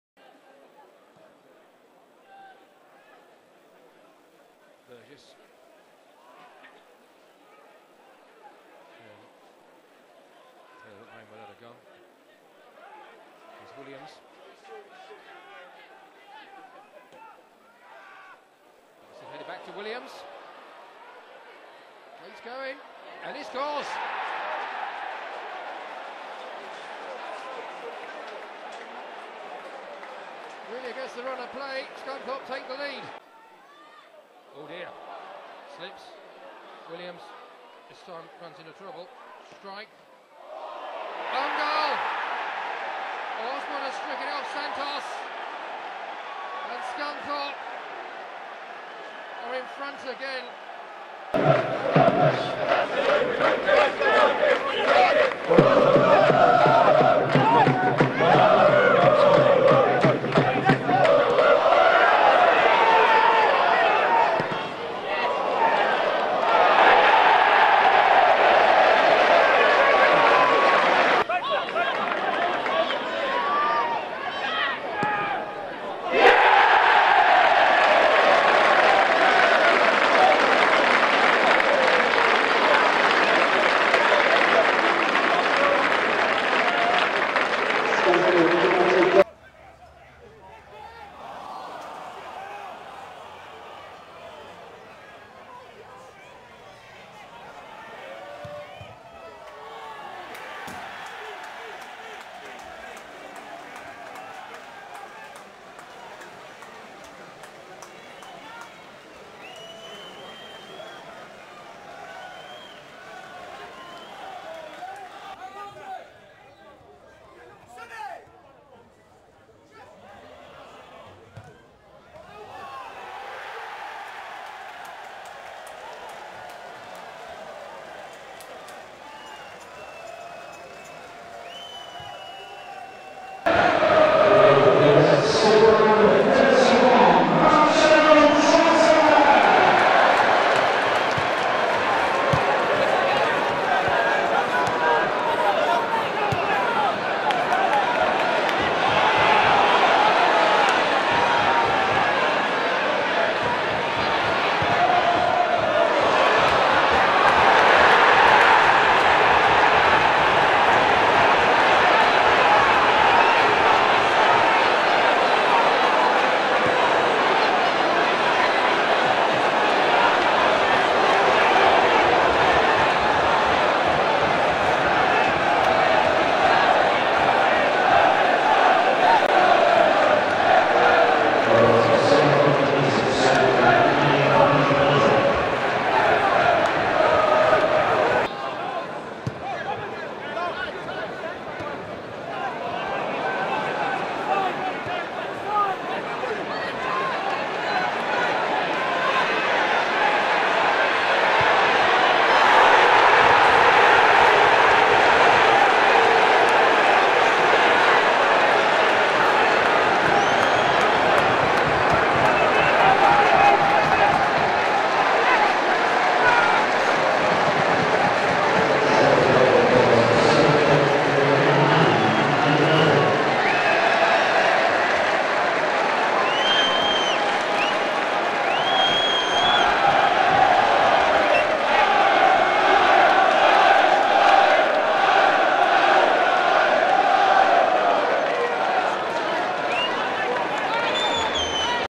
November Goal of the Month Voting
Vote for your November goal of the month before 12noon on Monday, December 8 and you could win a pair of Iron v Crewe match tickets.

The contenders are:
A) Luke Williams v Peterborough (A); November 1
B) Isaiah Osbourne v Peterborough (A); November 1
C) Gary McSheffrey v Forest Green Rovers (A); November 9
D) Gary McSheffrey v Forest Green Rovers (A); November 9
E) Paddy Madden v Crawley Town (A); November 22
F) Lyle Taylor v Crawley Town (A); November 22
G) Paddy Madden v Barnsley (A); November 29
H) Paddy Madden v Barnsley (A); November 29